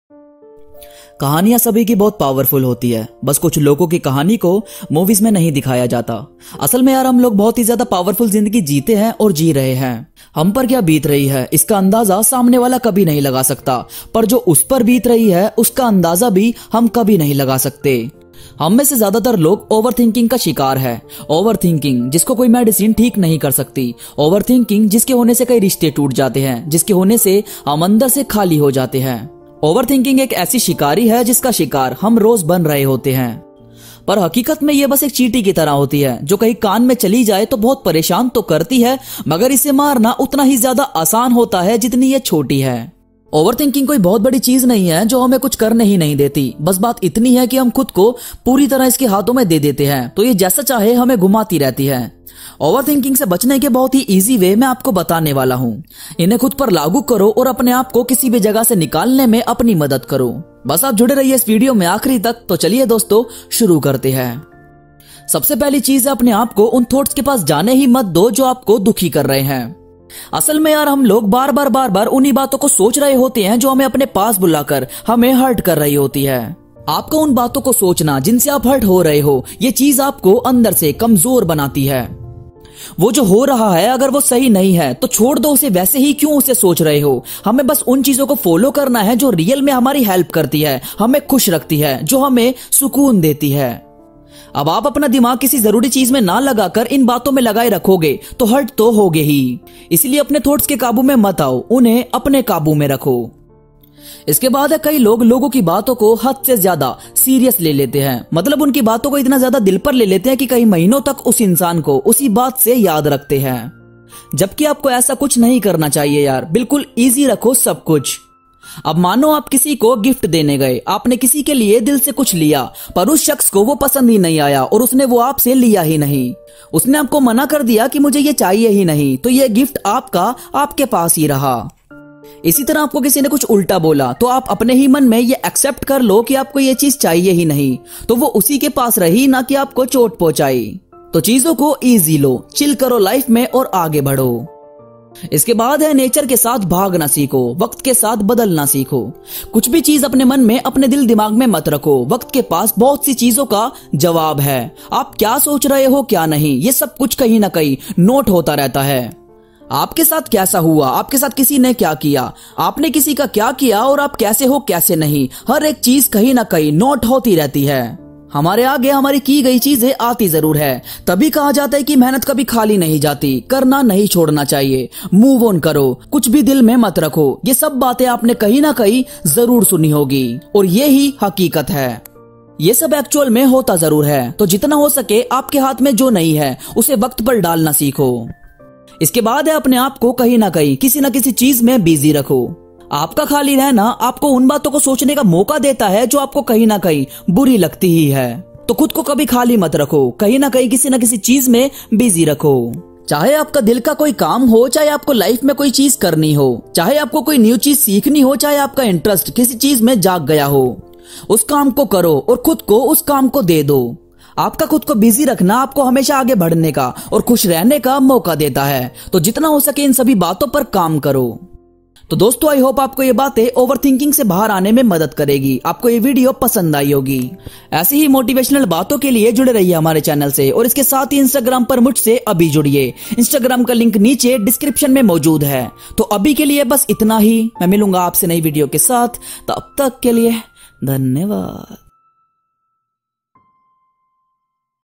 OVERTHINKING SE BAHAR NIKLE | Top Motivational quotes | best inspirational speech hindi
Namashkar Dosto

Aapko aaj ki video me janne ko milega ki overthinking se bahar kaise nikle
kaise hum overthinking se chhutkaara paa sakte hai